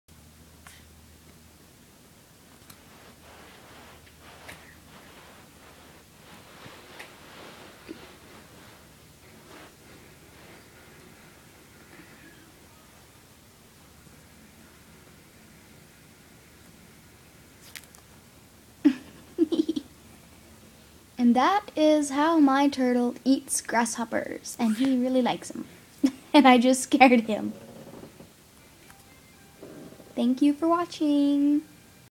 Herman the turtle grasshopper snack
My desert box tortoise, Herman, enjoys a healthy snack of a scrumptious grasshopper.